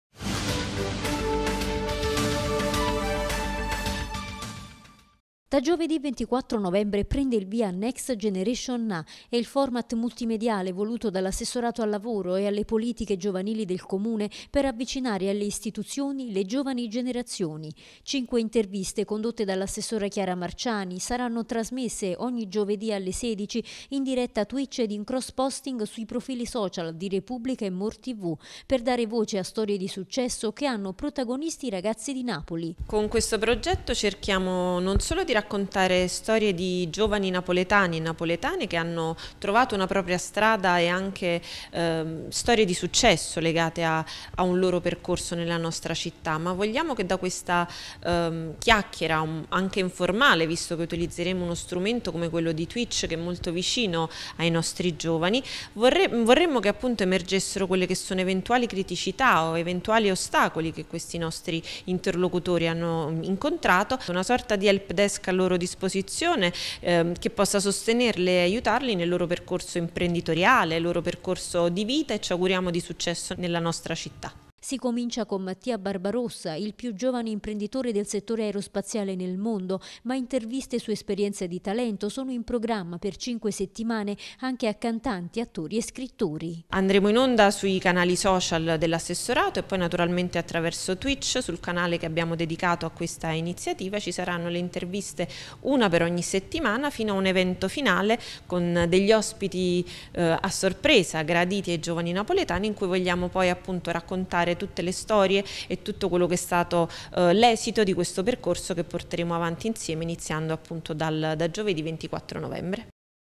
Next Generation Na i giovani di Napoli si raccontano su Twitch
Da oggi - giovedì 24 novembre ore 16 - al via un ciclo di interviste su Twitch condotte dall'assessore Chiara Marciani a giovani artisti e imprenditori di talento. E' il nuovo format 'Next Generation Na' ideato dal Comune per avvicinare le giovani generazioni alle istituzioni.